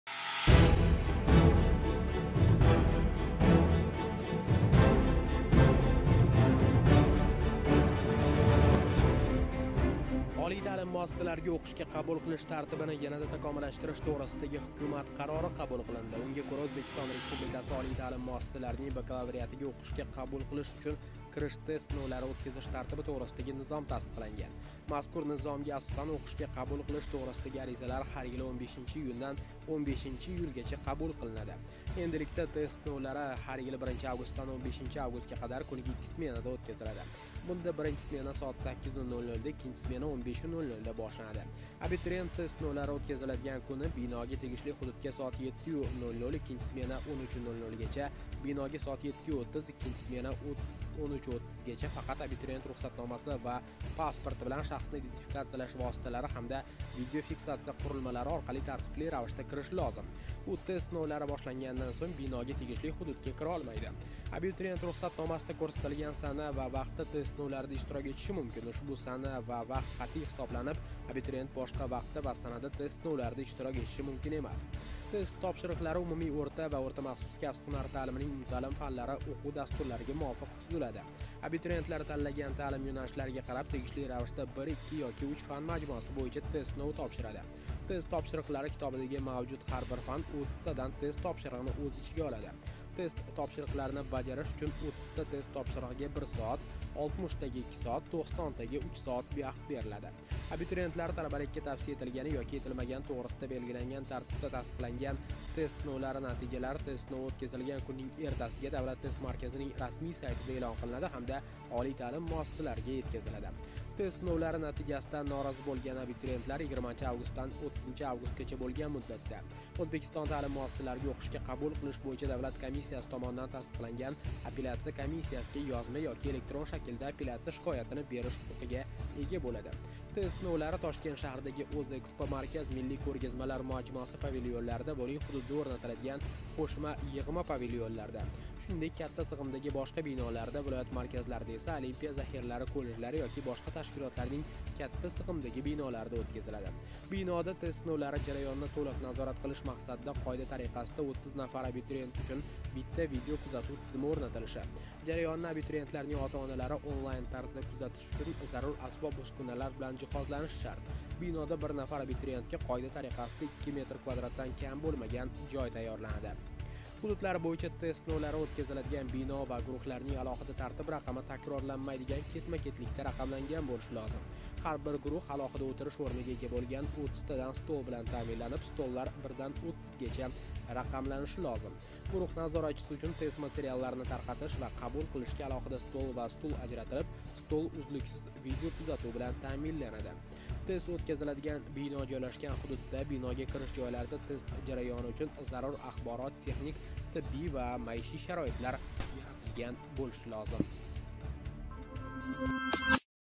BU YIL TEST VARAQASIDA 30TA SAVOL BO'LADI
Бу йил тест варақасида 30та савол бўлади. Кириш имтиҳонидаги асосий ўзгариш ва янгиликлар